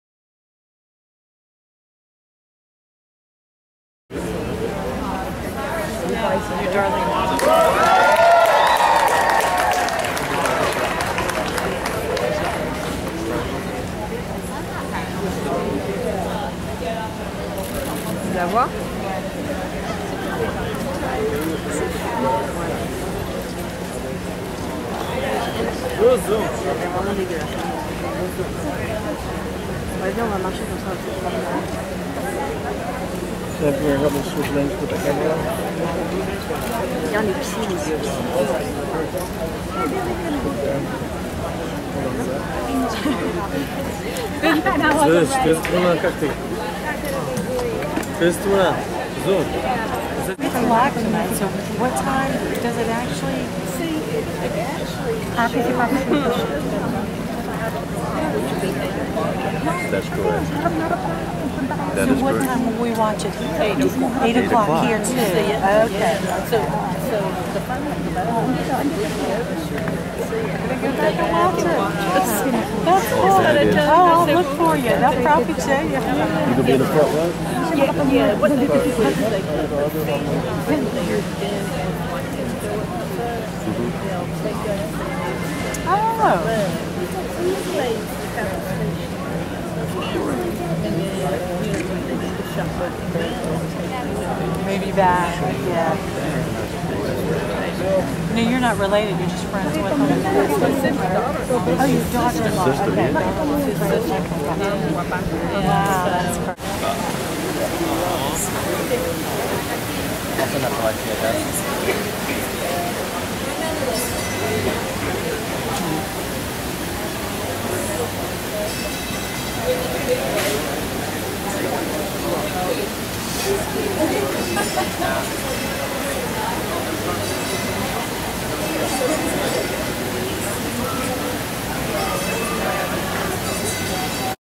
Paris Hilton and Kathy Interviewed at The Grove!
The moment you have all been waiting for, that's right folks! Paris and Kathy Hilton's Interview at The Grove! Follow us on Twiter for all the latest on Hollywood! We will tell you where to see Celebs! Twitter.com/HRVideos @HRVideos